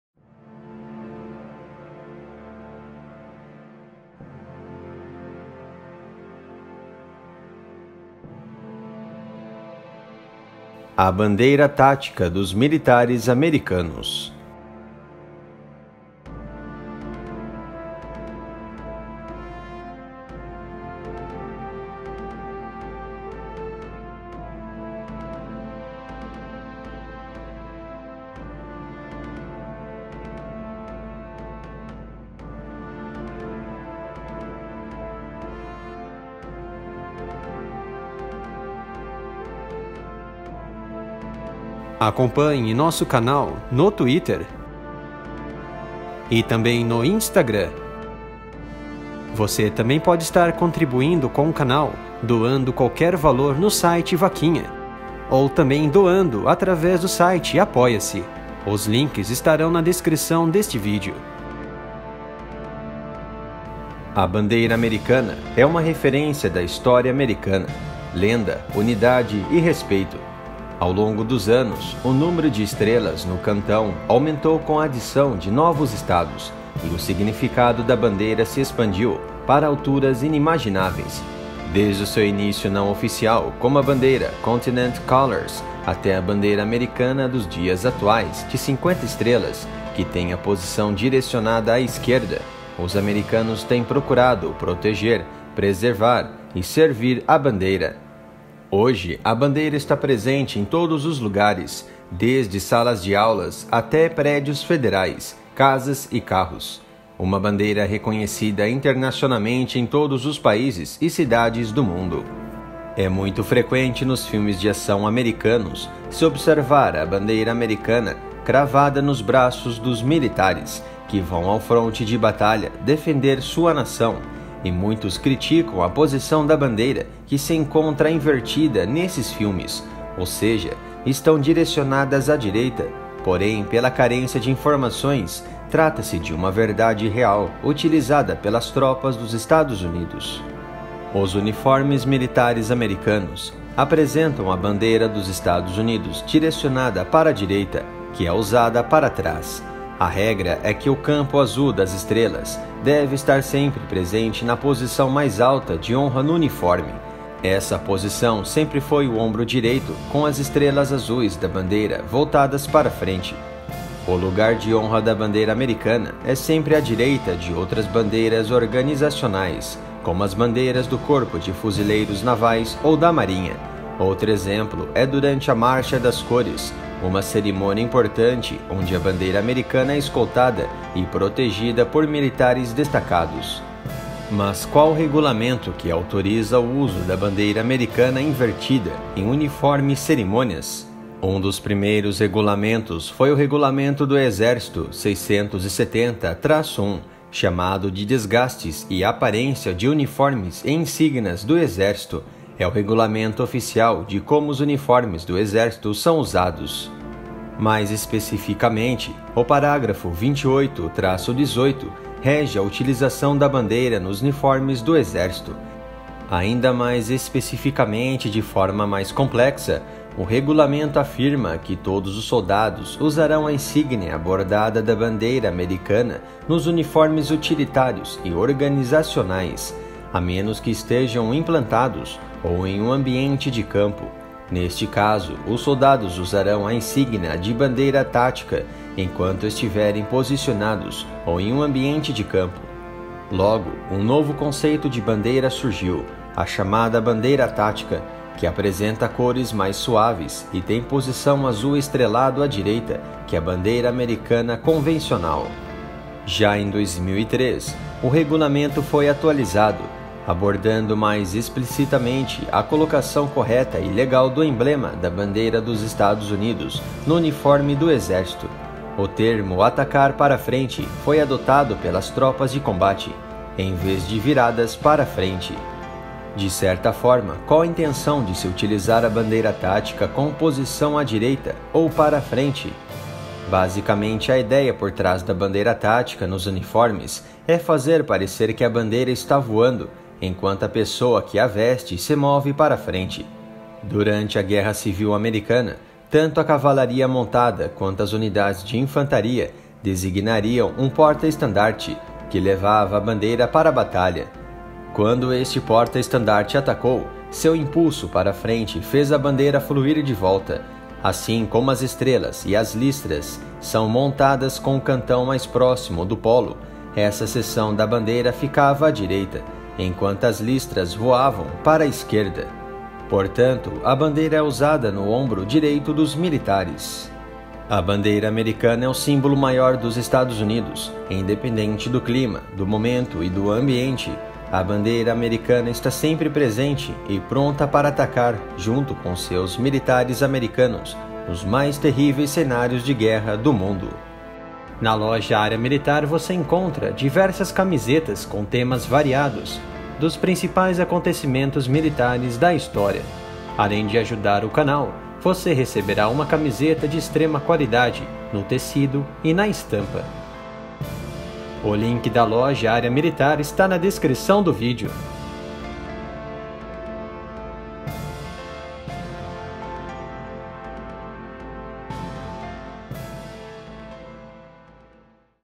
A Bandeira Tática Dos Militares Americanos
É muito frequente nos filmes de ação americanos se observar a bandeira americana cravadas nos braços dos militares que vão ao fronte de batalha defender sua nação, e muitos criticam a posição da bandeira que se encontra invertida nesses filmes, ou seja, estão direcionadas à direita, porém, pela carência de informações, trata-se de uma verdade real utilizada pelas tropas dos EUA.